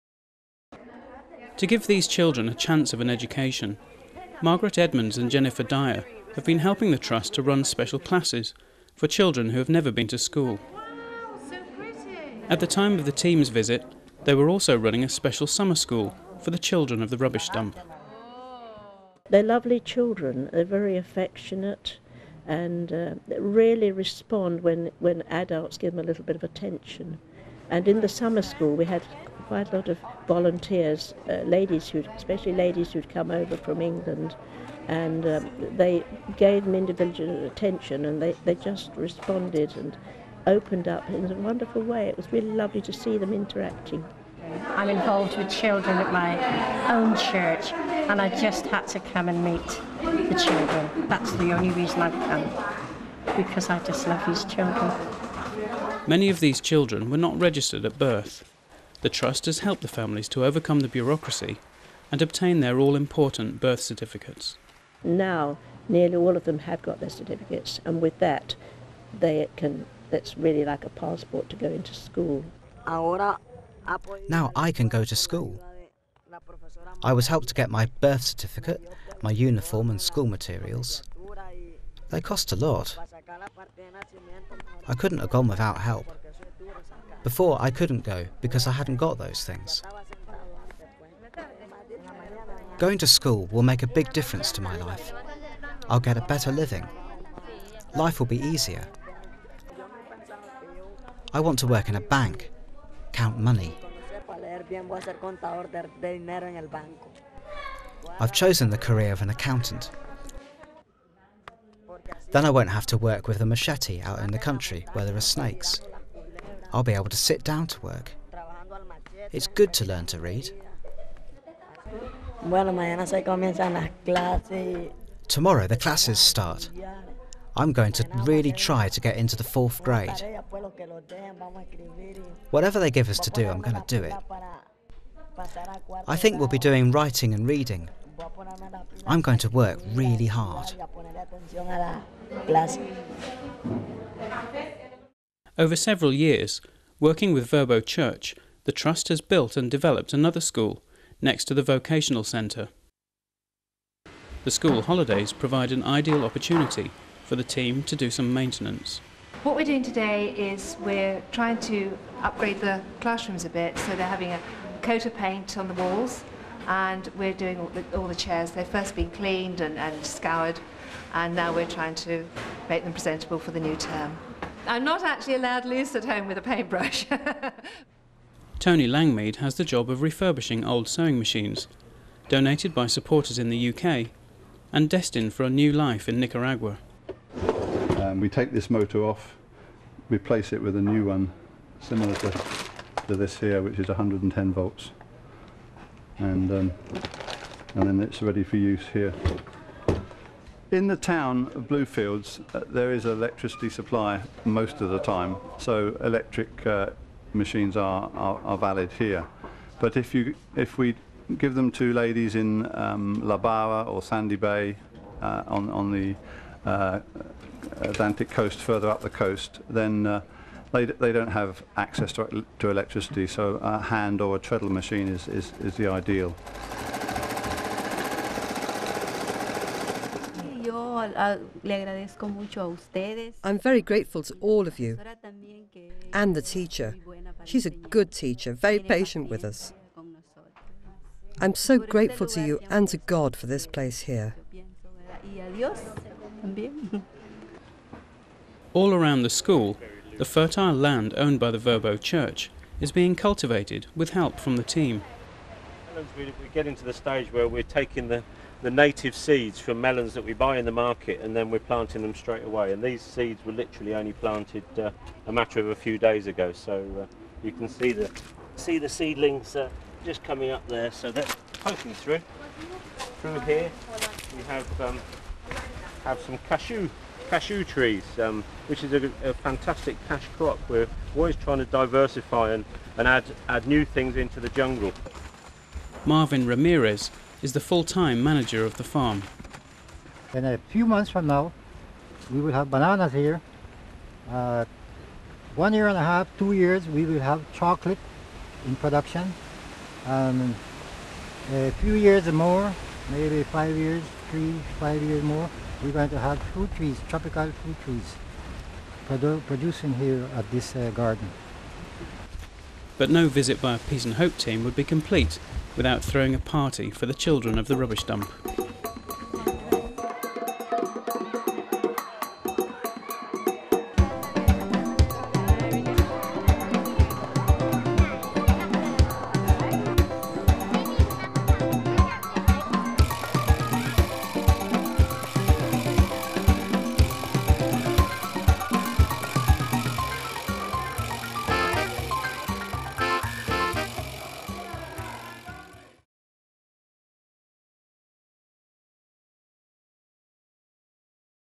Peace & Hope Trust: February 2006 Update [Part Two]
A film about the work of the Peace & Hope Trust in Nicaragua.